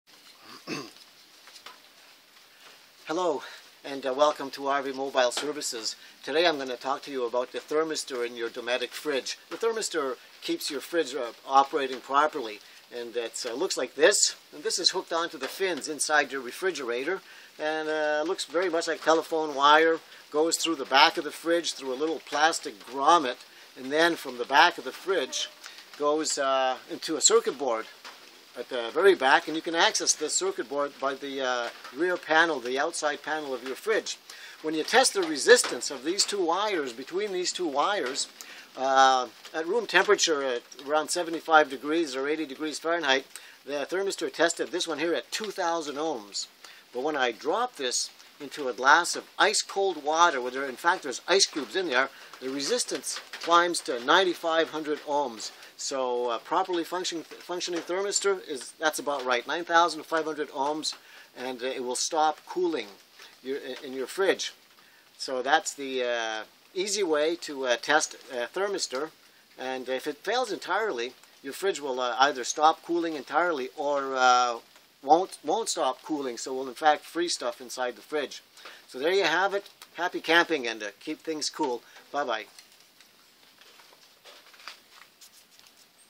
Dometic Thermistor how to test
How to test the thermistor in your Dometic fridge, the easy way, using an ohmmeter.